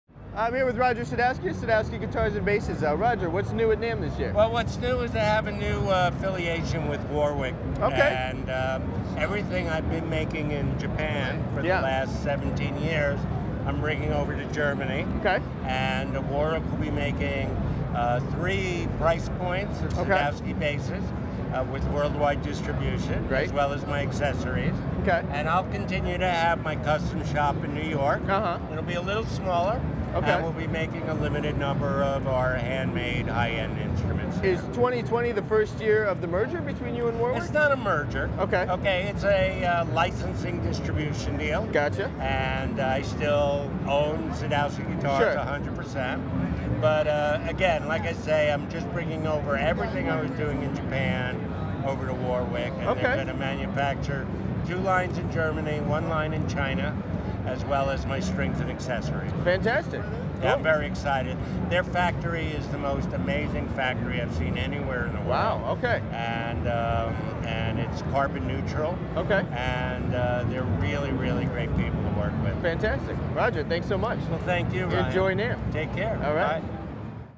Roger Sadowsky
Ryan Roberts talks with Roger Sadowsky at NAMM 2020